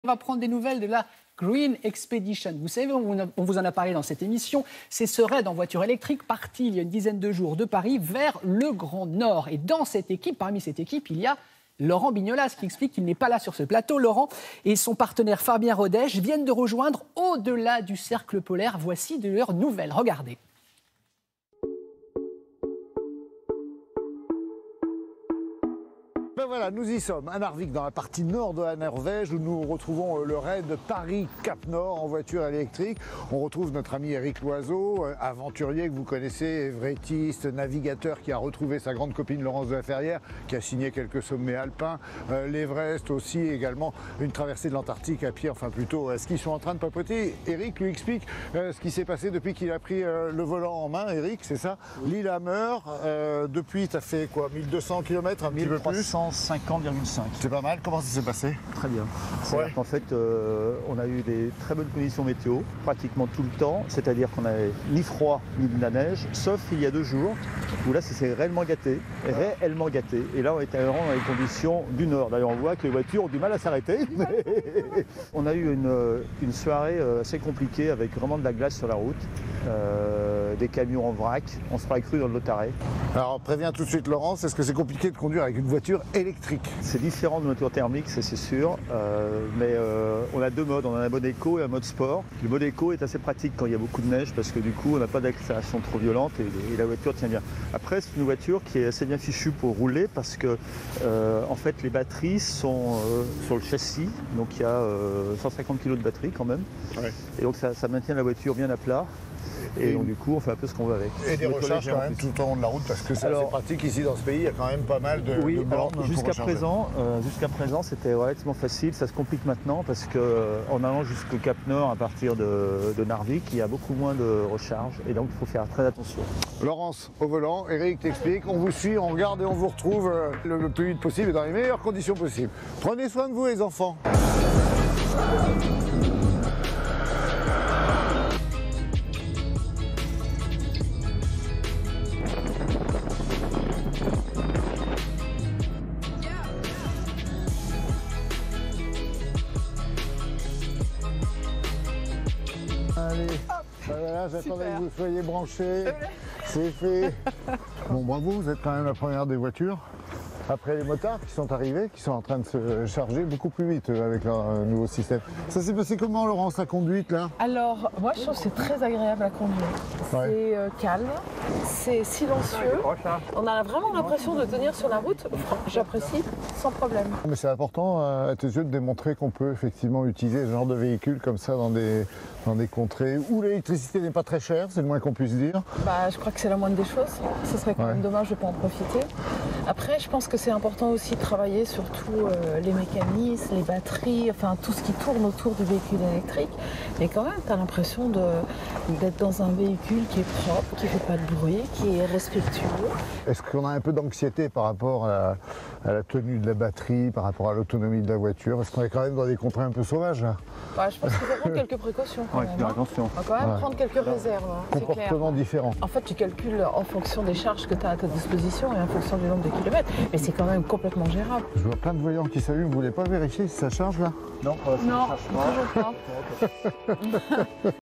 Raid Cap Nord
Chroniqueur Laurent Bignolas

La « green expedition », c’est un rallye 100% électrique qui roule direction le Cap Nord.
Laurent Bignolas était au départ de la course, il nous envoie des cartes postales de ses aventures.